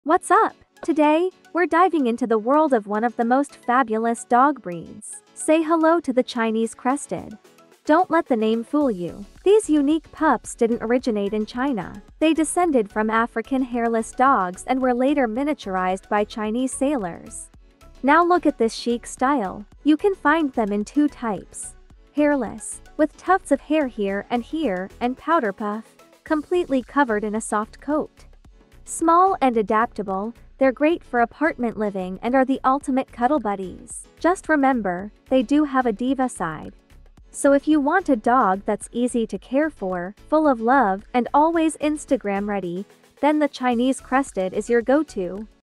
Meet the Diva of Dog Breeds: The Chinese Crested! 🐶💖✨
Unveiling the fascinating history, fabulous looks, and perfect fit of the Chinese Crested breed. Is this Instagram-ready 📸 pup your next fur baby? 🐾💫